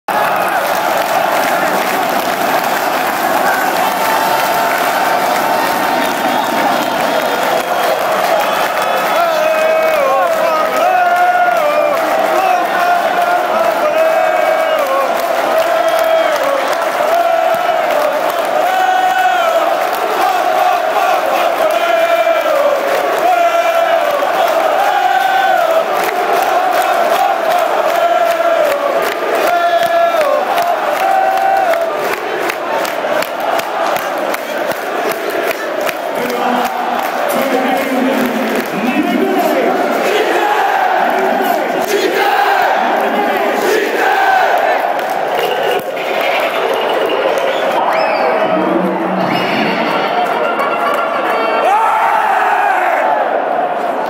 Standard-Anderlecht 2-1 14/5/2011
Ambiance après le deuxième but de Tchite